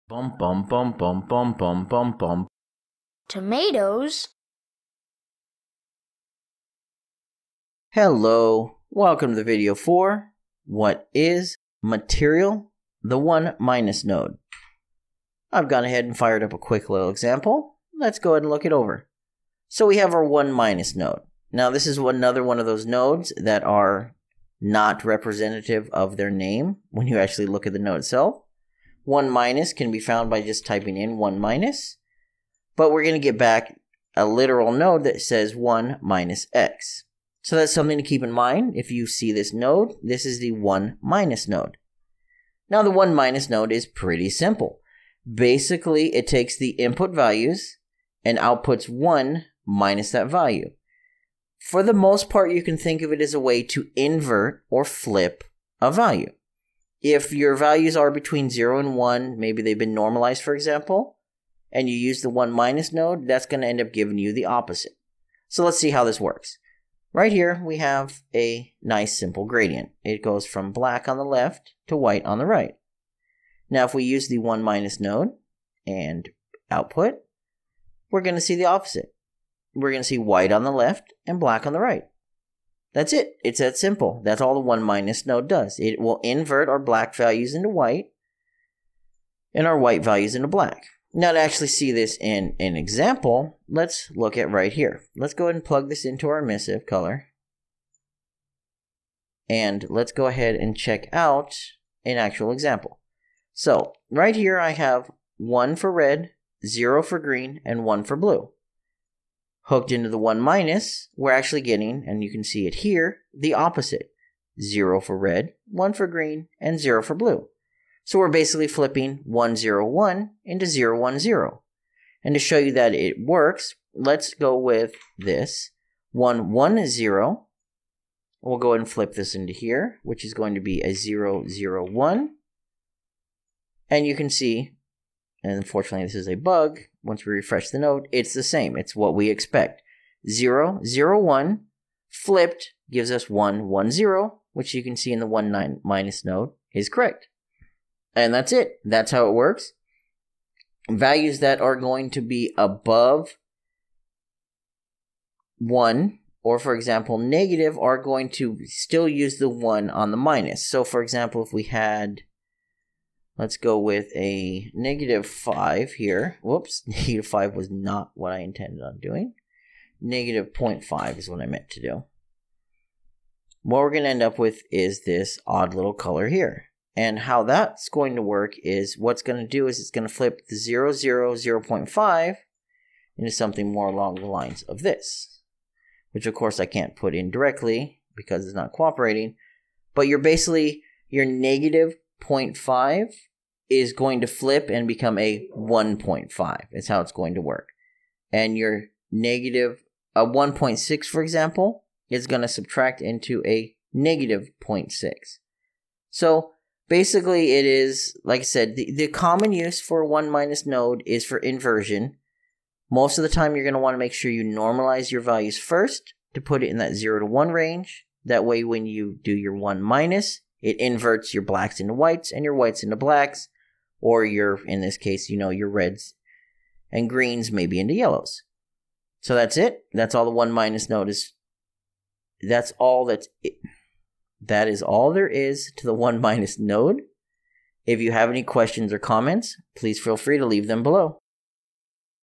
WTF Is? Material - One Minus in Unreal Engine 4 ( UE4 )
What is the Material: One Minus Node in Unreal Engine 4